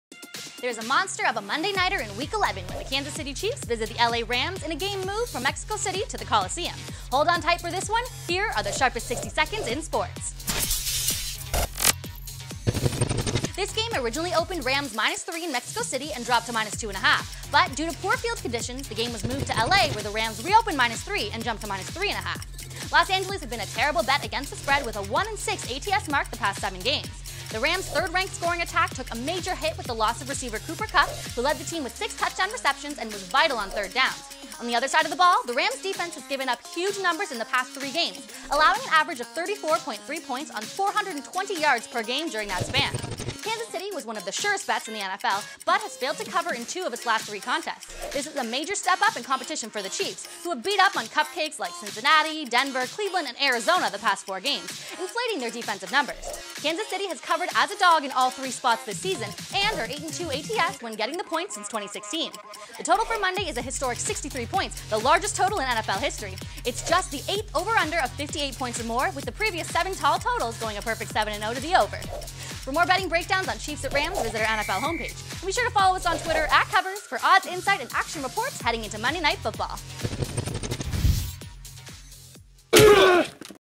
Sharpest 60 Seconds In Sports: Chiefs versus Rams is breaking NFL betting records
There’s a monster of a Monday nighter in Week 11 when the Kansas City Chiefs visit the L.A. Rams in game moved from Mexico City to the Coliseum. Hold on tight for this one, here are the “Sharpest 60 Seconds in Sports”.